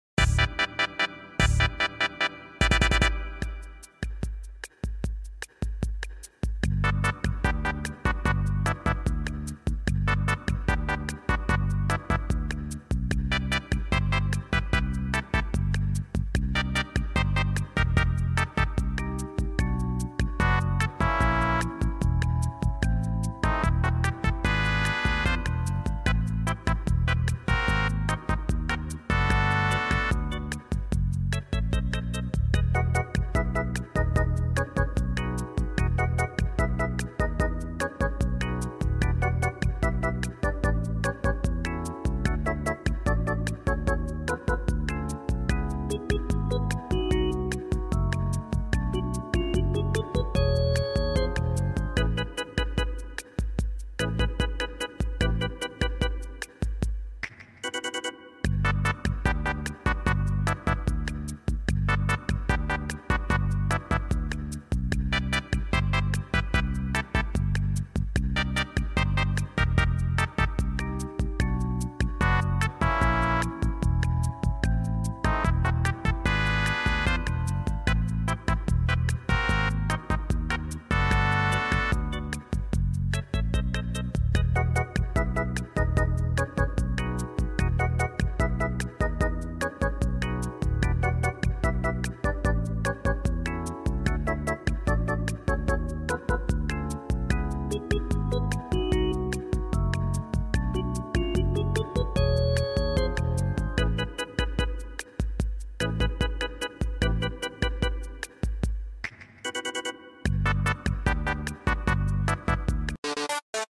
how 2 draw stickman [100 sub special]
yes I teach you how to draw stickman, get your drawing pad or mouse ready!!